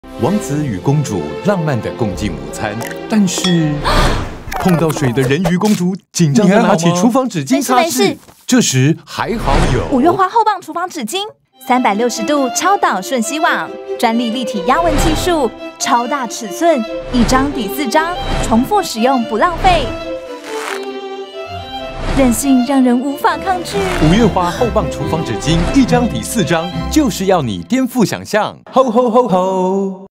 五月花厚棒廚房紙巾_海洋傳說篇
首次約會就把水打翻！手忙腳亂擦不乾 好糗😰
瞬吸水分！料理、清潔、生活一紙完勝！

五月花 📣 厚棒廚房紙巾 重「棒」級登場📣
⭕ 專利立體壓紋技術，360度超導瞬吸網
⭕ 紙張厚實強韌，超大尺寸1張就夠
⭕ 耐用不易破，水分擠乾再使用💯
1張抵4張，強效吸水解危機👌

全聯、家樂福 熱賣中